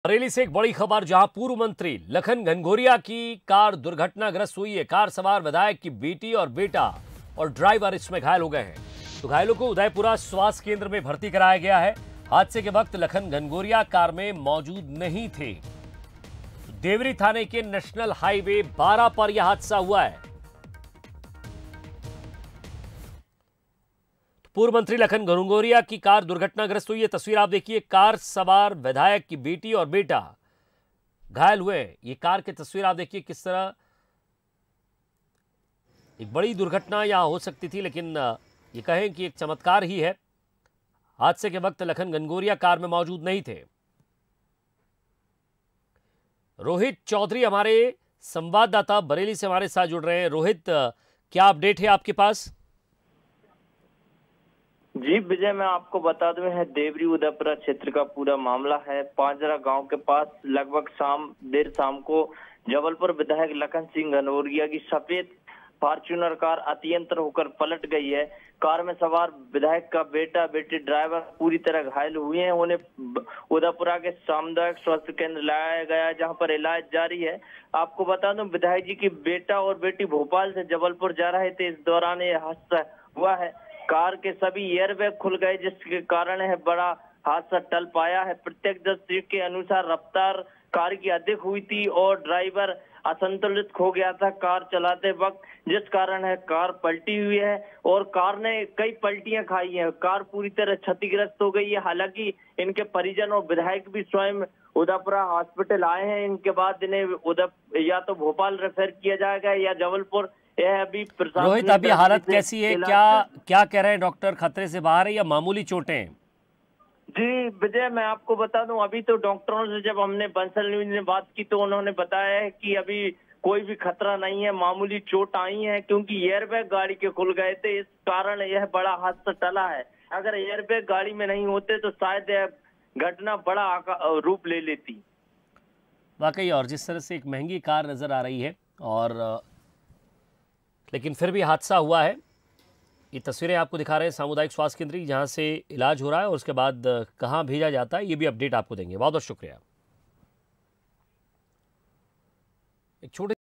पूर्व मंत्री लखन घनघोरिया की कार दुर्घटनाग्रस्त, विधायक की बेटी, बेटा और ड्राइवर घायल
पूर्व मंत्री लखन घनघोरिया की कार हुई दुर्घटनाग्रस्त
कार सवार विधायक की बेटी, बेटा और ड्राइवर घायल
घायलों को उदयपुरा स्वास्थ्य केंद्र में कराया भर्ती
हादसे के वक्त लखन घनघोरिया कार में नहीं थे मौजूद
देवरी थाने के NH-12 पर हुआ हादसा